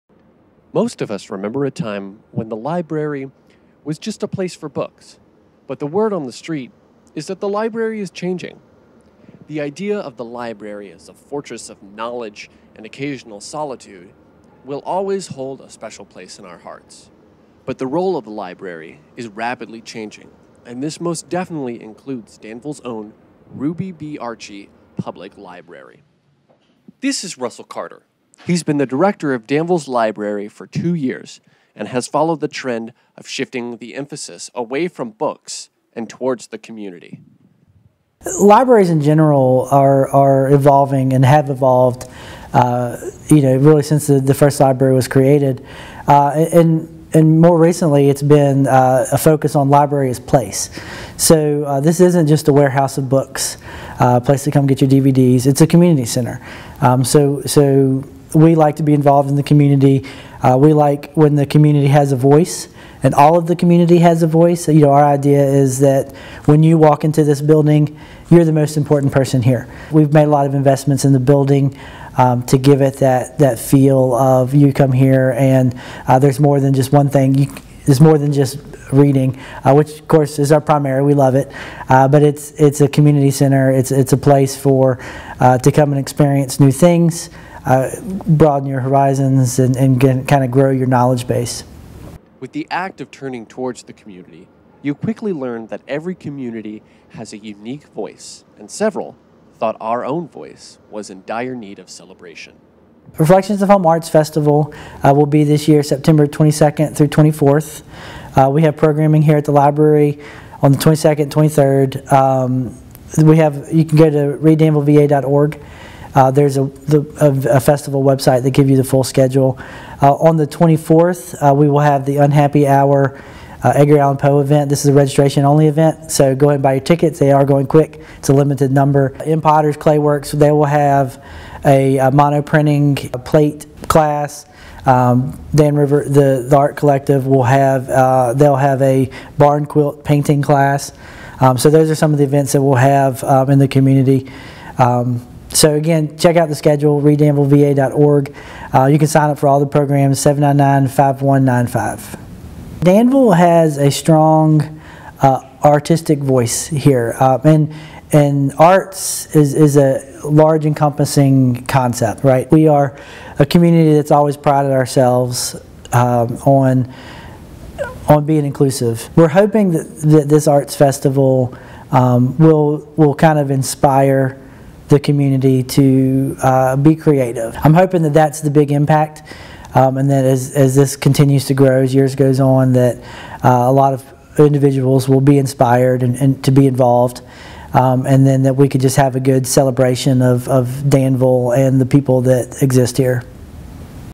Reflections of Home Arts Festival - Interview with Russell Carter
Join us as we sit down with Russell Carter, director of the Ruby B. Archie Public Library and discuss the upcoming reflections of Home Arts Festival